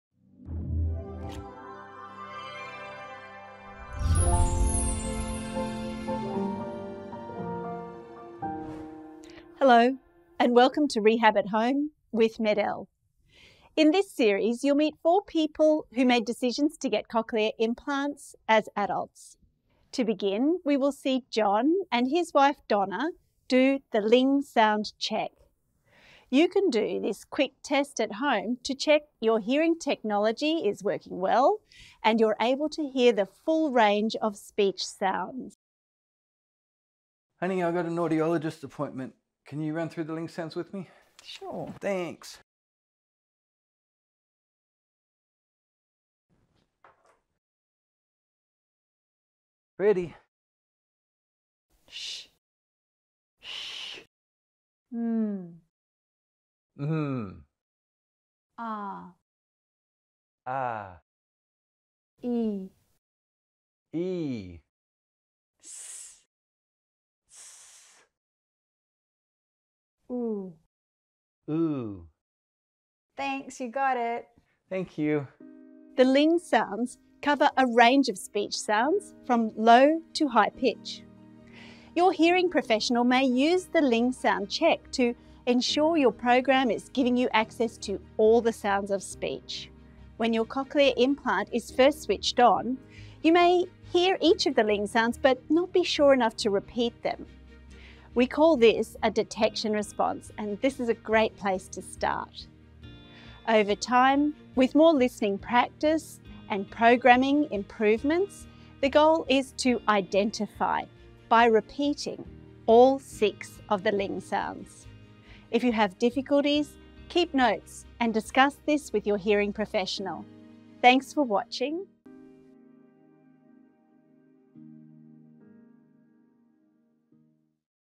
Sons do Ling | Reabilitação em Casa
Procurando por atividades de reabilitação auditiva para adultos que possa fazer em casa? Vamos começar a publicar agora diversos vídeos da série Reabilitação em Casa para que possa acompanhar.

Neste primeiro episódio, nossa especialista Rebecca fala sobre o Teste de Sons de Ling, um tipo de teste que cobre todo a faixa de sons da fala, do mais grave ao mais agudo. Neste vídeo você irá ver o usuário de implante coclear John demonstrar o teste.

Você pode fazer o teste rapidamente em casa para verificar se seu dispositivo está funcionando bem se consegue ouvir todas as frequencias de som da fala.

Fique ligado para os próximos episódios!

Já conhecia esse teste?